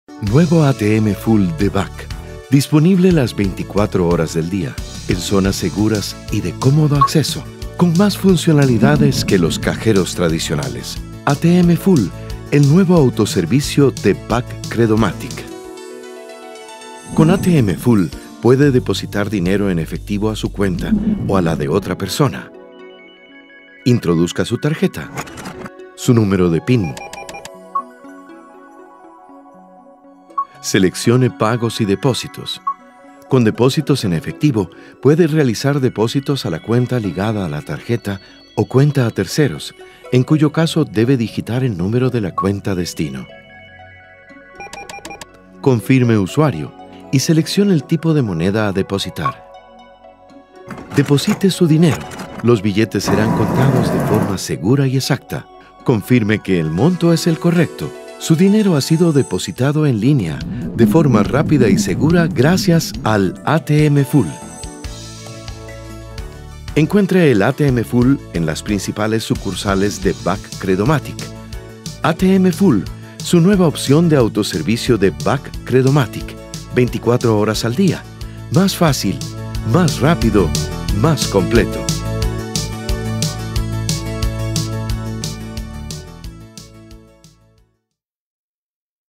Nicaragua Déposito en Efectivo
Deposite su efectivo en cuentas propias o de terceros fácilmente en los cajeros ATM Full de BAC-Credomatic en Nicaragua.

Cajeros ATM Full disponibles para depositar su efectivo en cajeros de Managua, Chinandega, Granada, Rivas, Masaya, León.